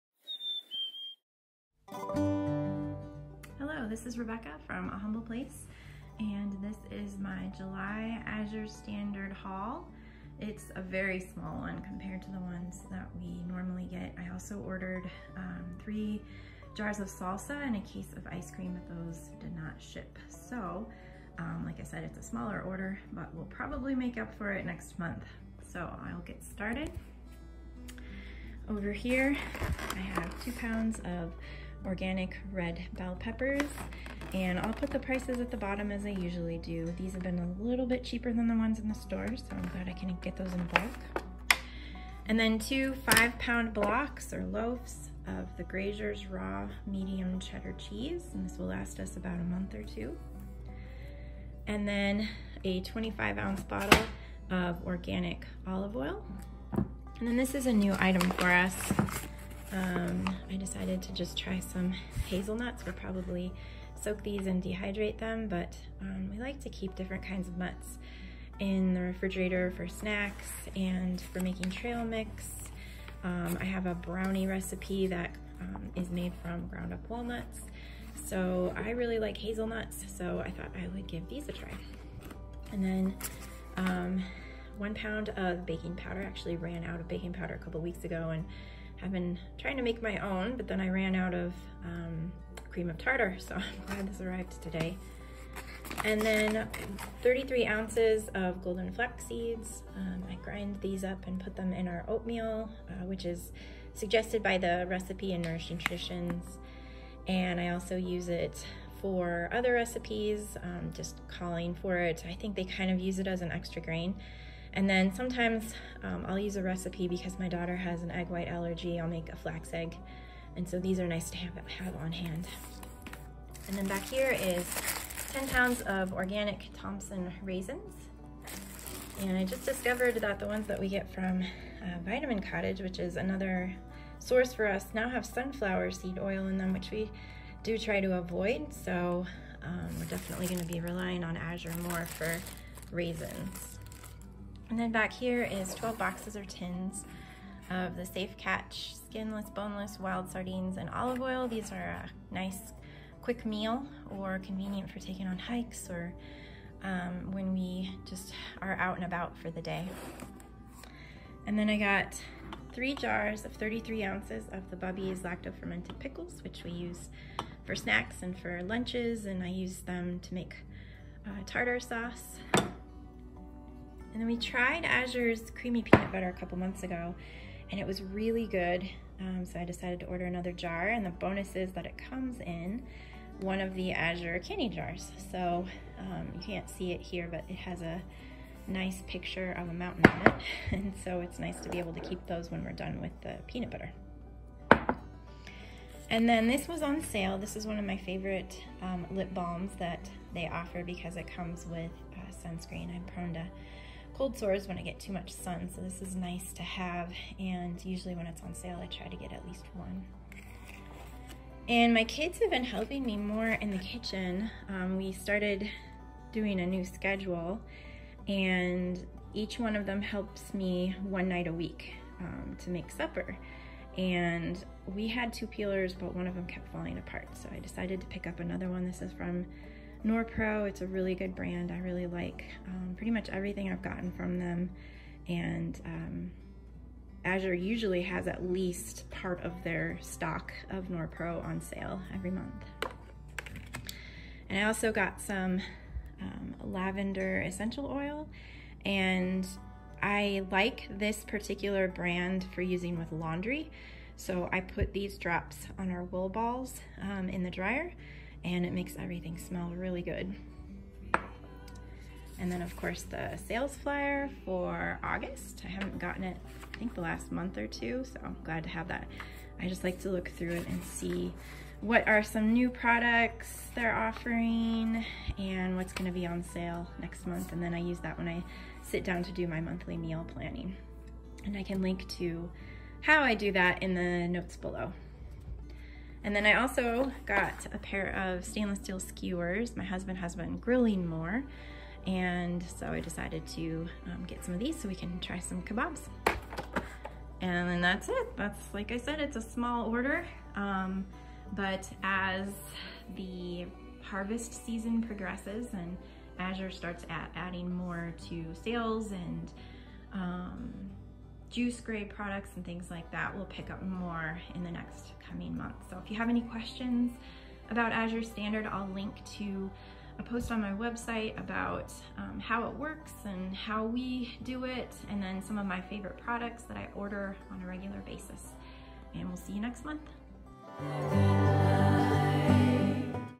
Azure Standard Haul - July 2024 (with prices!)
Definitely getting this up late, but it was a busy summer!

Our favorite things to buy at Azure Standard and how it works

How I do my monthly meal planning (with Ancestral Kitchen Podcast and Plan To Eat Podcast interviews)